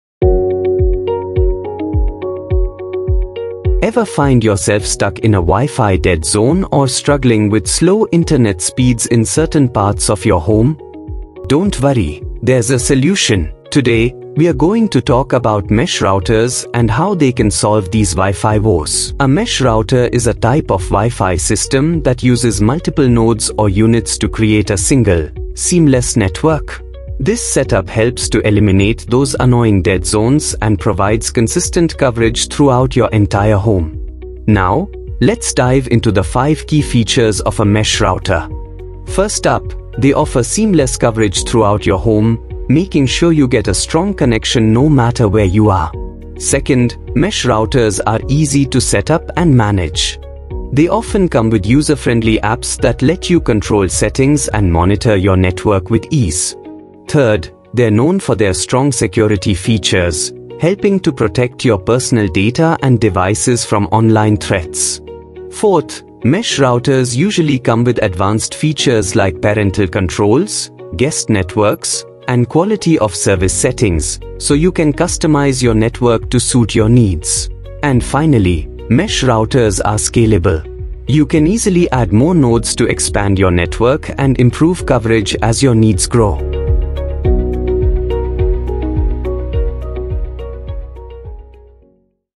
Mesh Wi-Fi: What is Mesh Wi-Fi Router | ACT Fibernet Smart Wi-Fi
Struggling with weak Wi-Fi signals and dead zones? Unlike traditional routers or range extenders, mesh routers use multiple nodes to create a unified network. This ensures seamless, high-speed internet across your home without interruptions or manual network switching. Let ACT Zippy guide you to a smarter, more connected home!